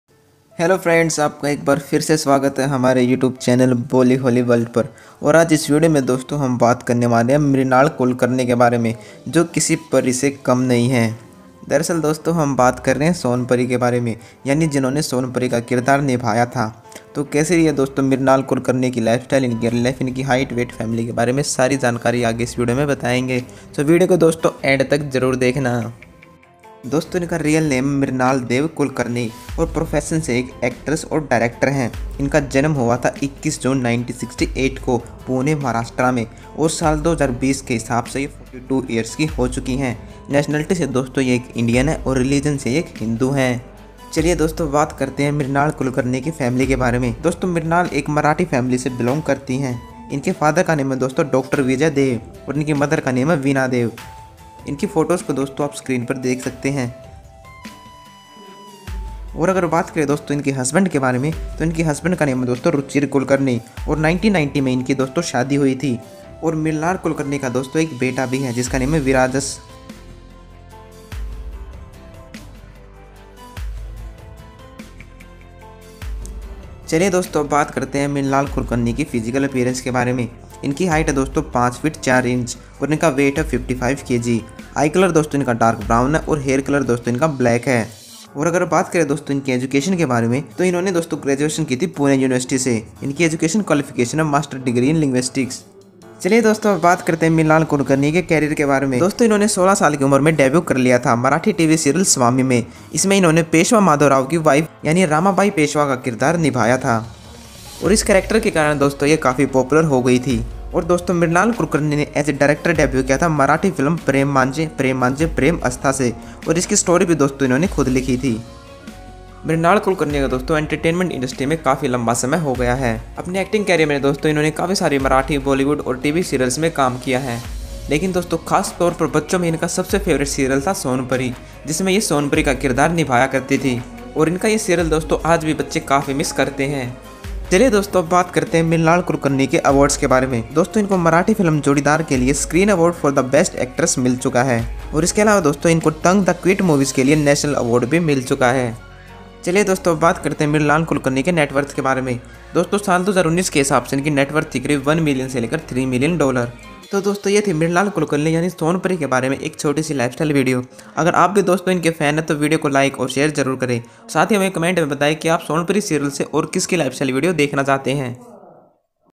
Mrinal Kulkarni (Son Pari) Lifestyle 2020, Age, Family, Husband, Son, Serials, Real life & Biography
Mrinal Dev-Kulkarni (born 21 June 1968)[2] is a film and soap opera actress and director in India known for her role in popular Hindi language TV serial Son Pari. She has acted in both Marathi and Hindi TV serials and films